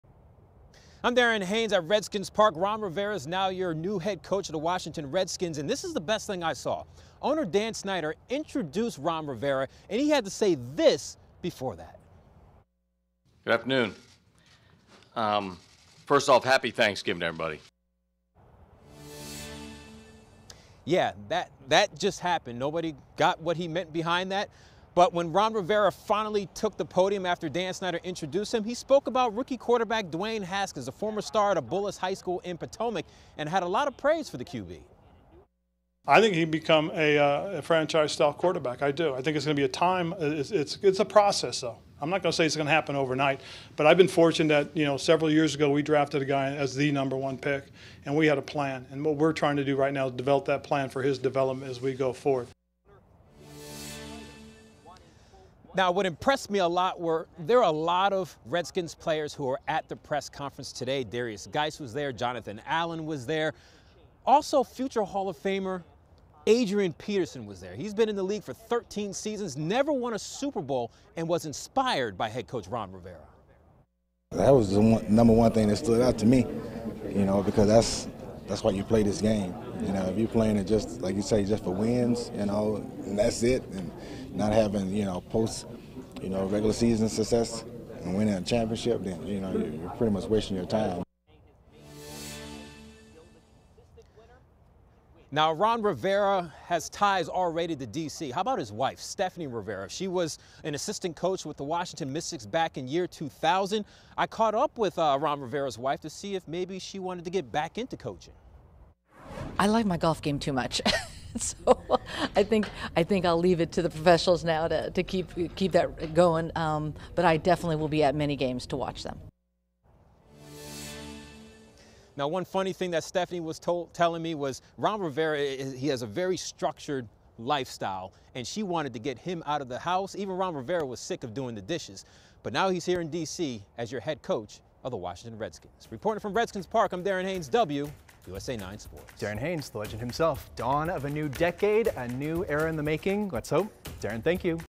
Washington Redskins enter into a new era
It's the beginning of a new era at Redskins Park tonight. Sports Director Darren Haynes has the latest.